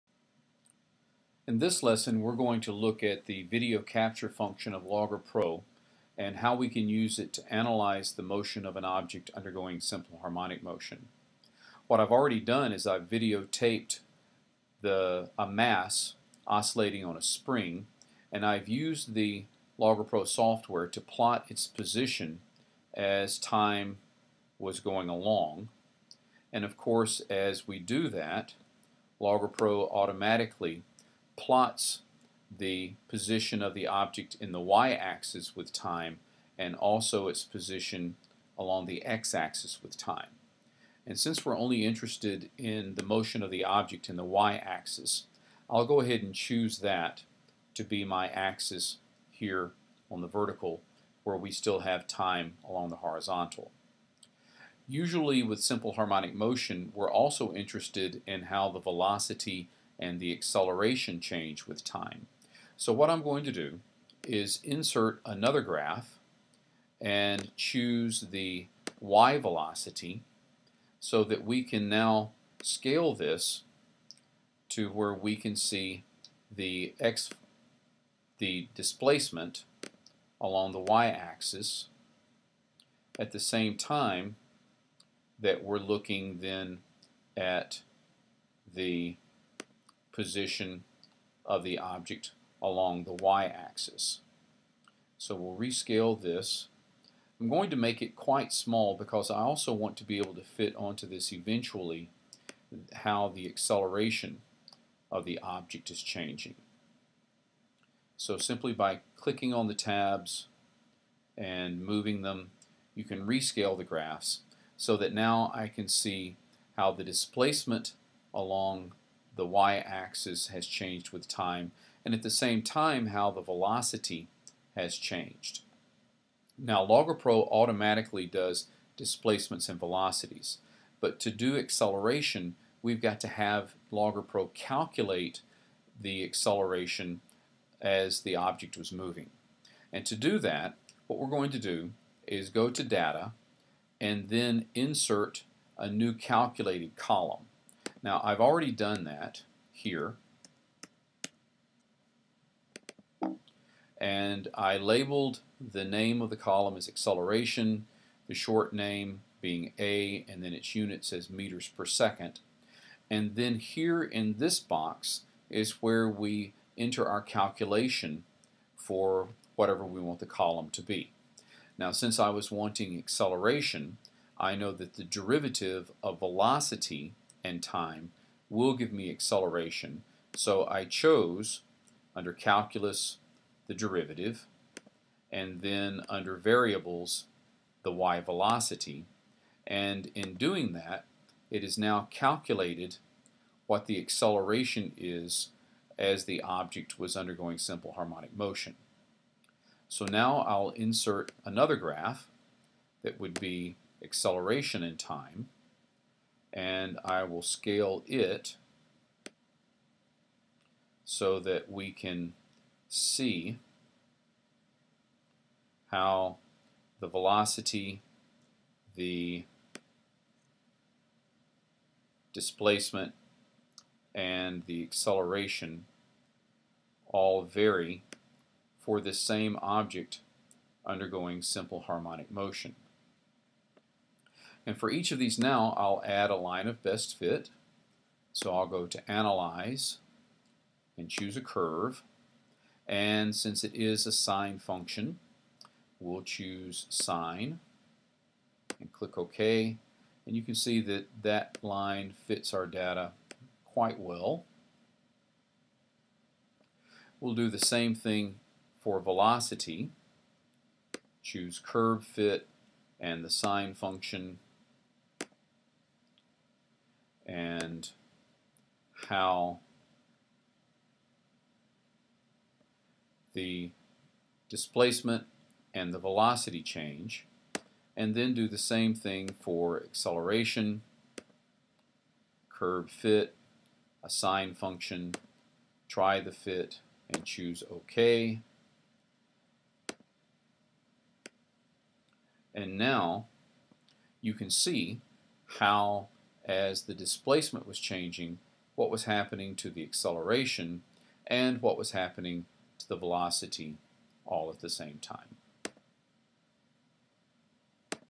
Analysis of simple harmonic motion
Using video capture to analyze simple harmonic motion, looking at displacement, velocity and acceleration graphs all at once.